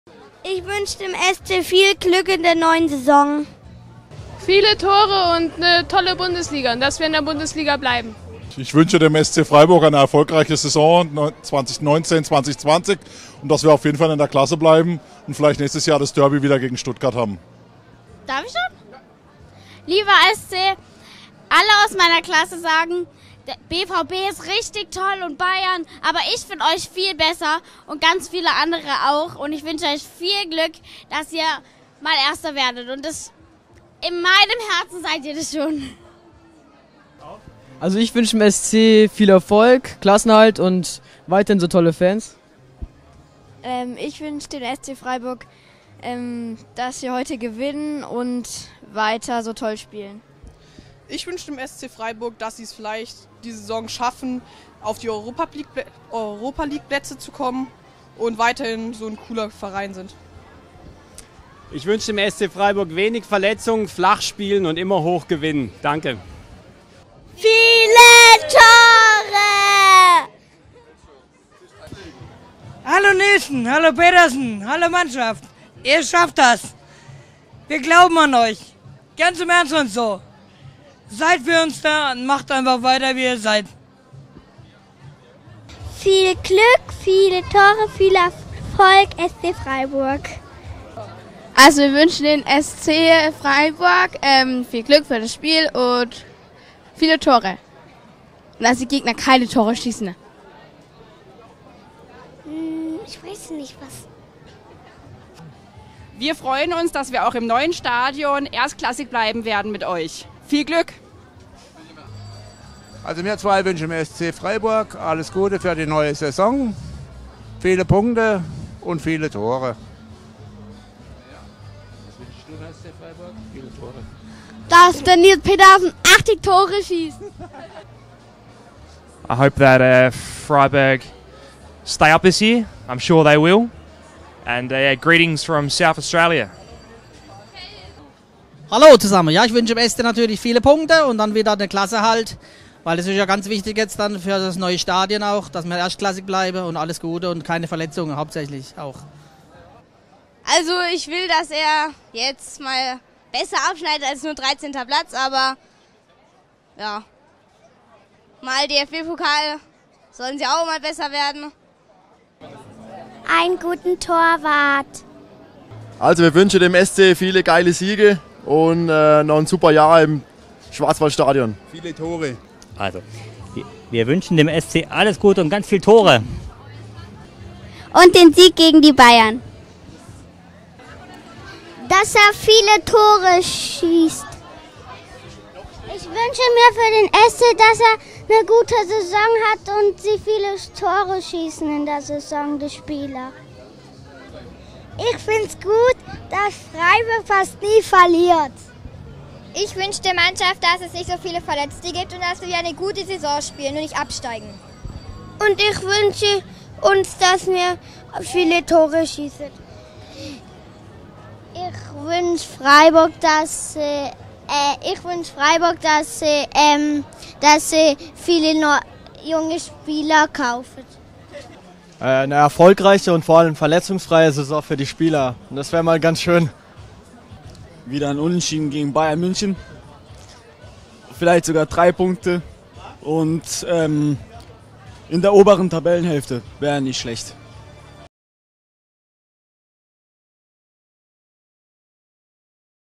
Das wünschen sich Fans des SC Freiburg für die neue Saison
Viele Tore, wenig Verletzungen, Meisterschaft, Klassenerhalt: Am 17. August startet der SC Freiburg gegen Mainz 05 in die Saison. Was wünschen sich die SC-Fans im Frühjahr?